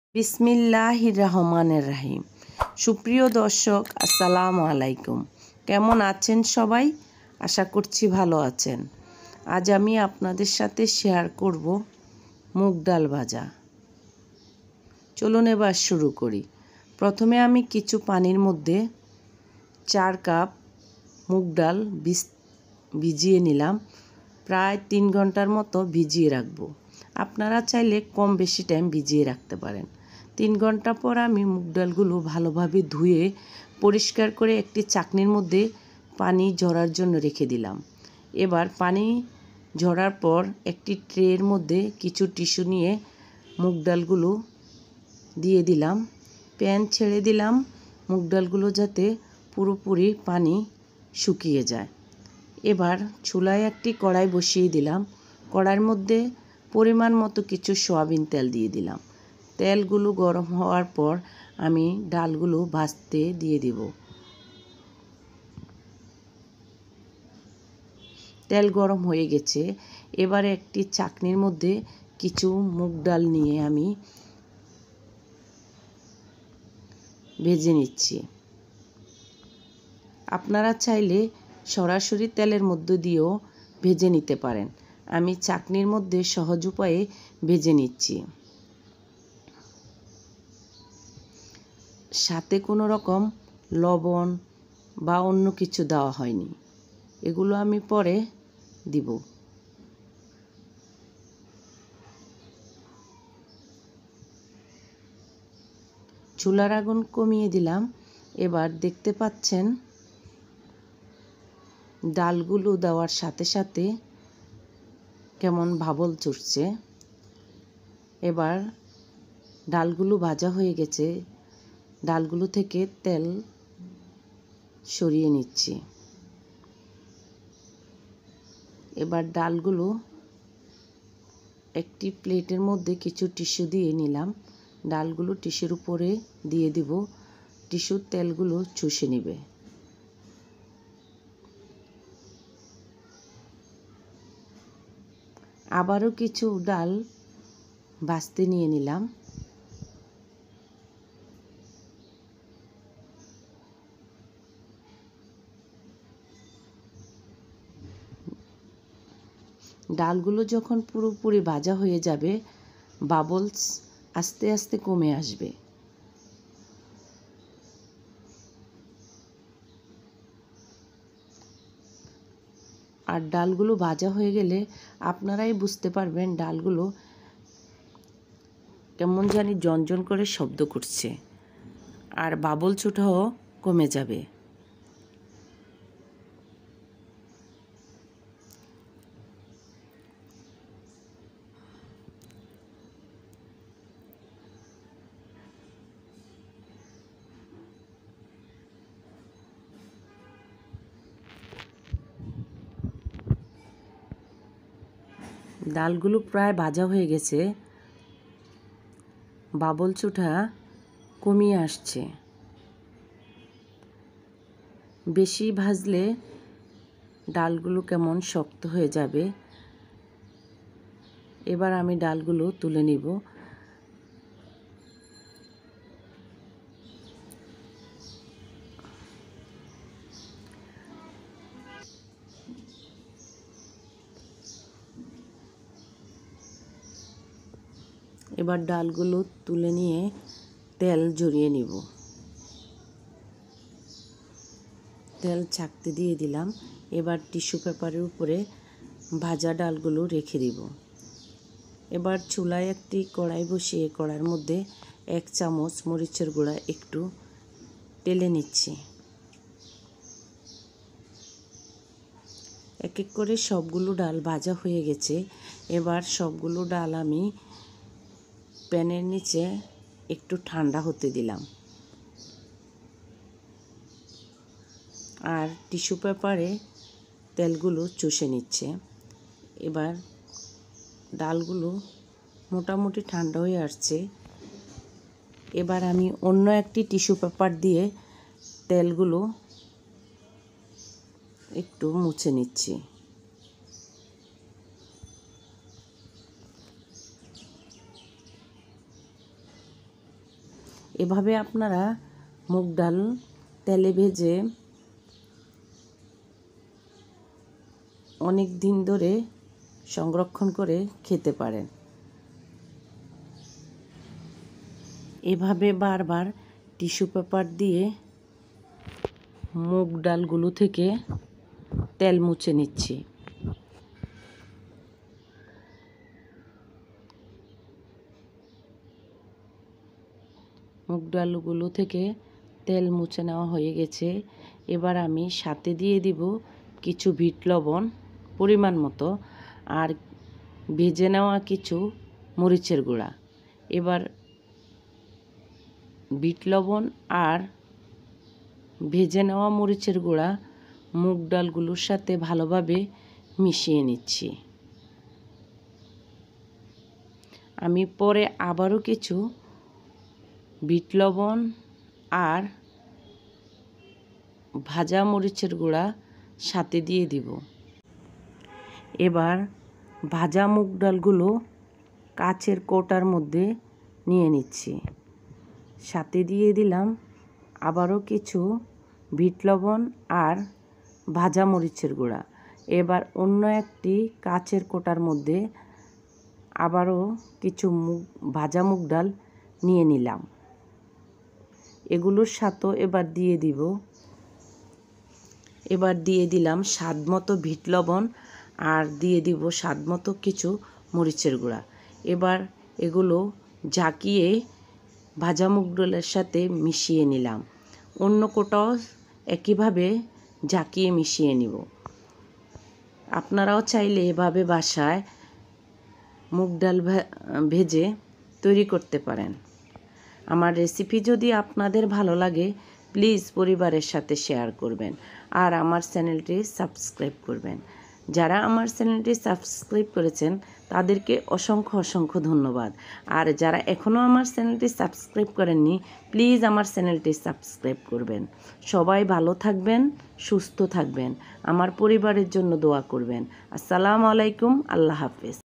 Crispy Moong Dal bhaji|||Momotaj's Cooking with Vlogs
Ingredients :

Moong dal
water
oil
bit salt
chili powder

🌟Social Media🌟